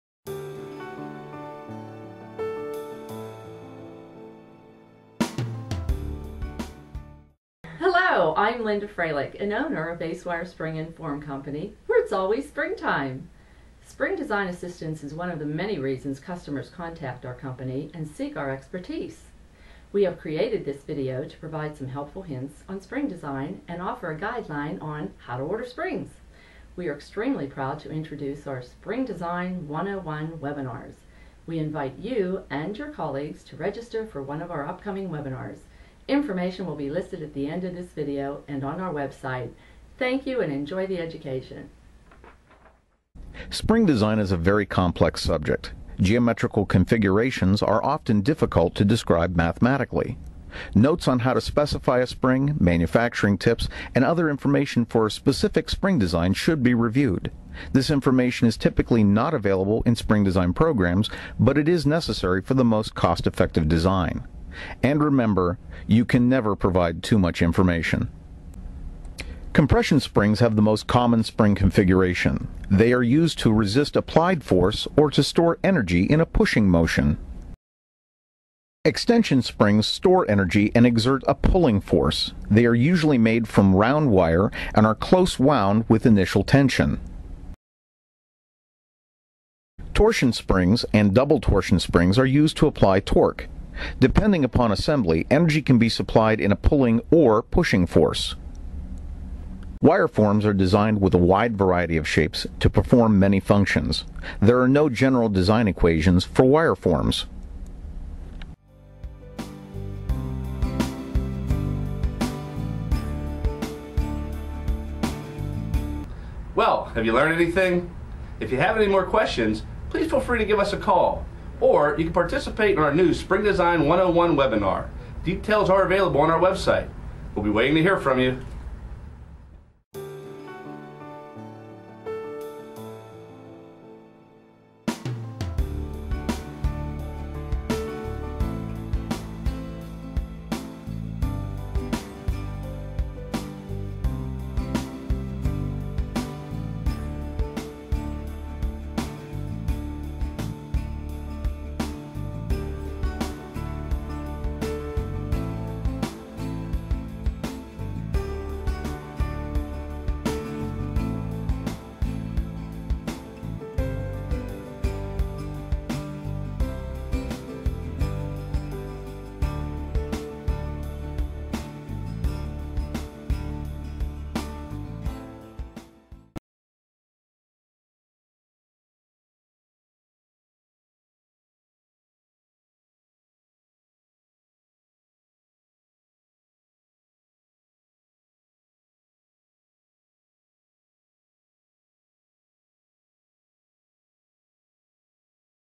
Spring Design
Spring Design is an educational video detailing the information necessary for specifying and designing a spring. Ace Wire Spring is also introducing a Spring Design Webinar. Visit our web site for more information on Spring Design